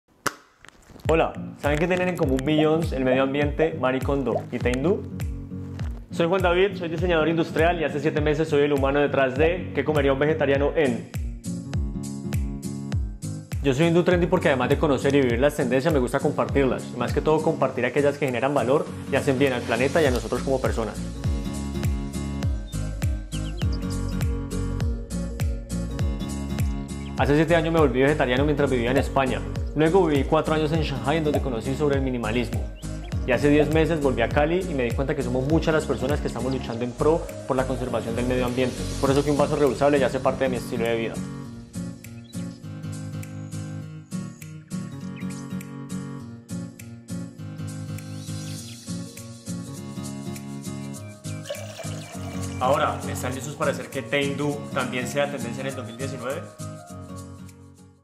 Juan David González QCUVE
Soy Juan David y seré el próximo Hindu Trendy! ✌🏻😎

Pueden conocer un poco más sobre mi en Instagram @quecomeriaunvegetarianoen y @juandavidgonzalez28